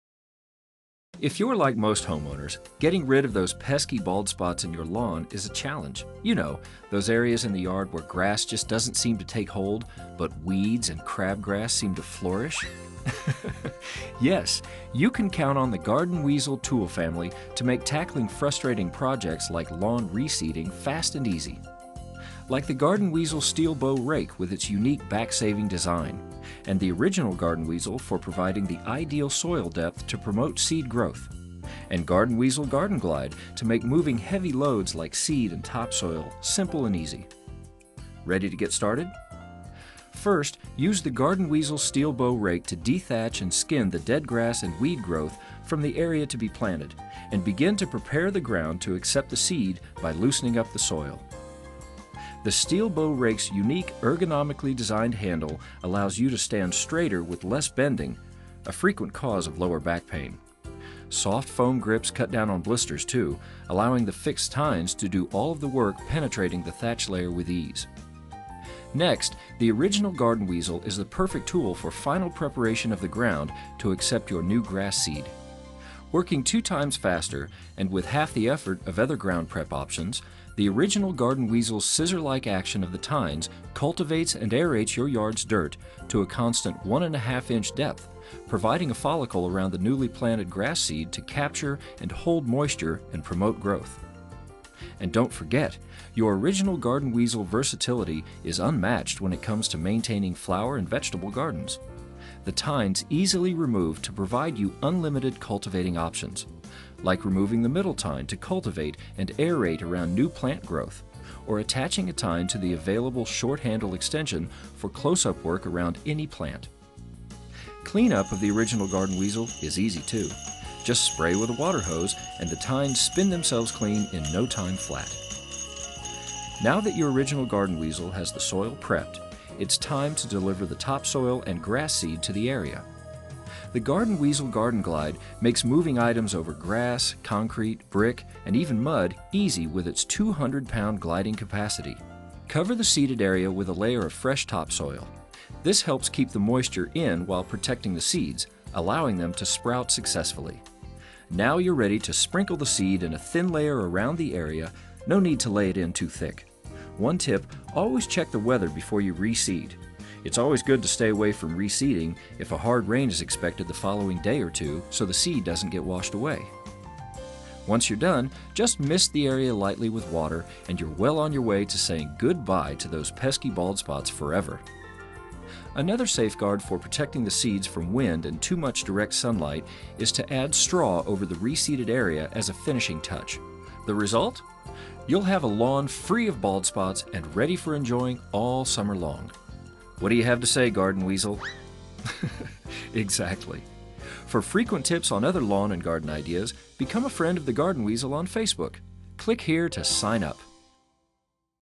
Garden Weasel Yard & Lawn Seeding, Reseeding & Repair
Quick how-to on reseeding your lawn using Garden Weasel's innovative line of gardening tools, such as our Garden Weasel Steel Bow Rake, the Original Garden Weasel, and the Garden Weasel Garden Glide.

For more tips on our tools and putting them to work for you in your lawns and gardens, join us on Facebook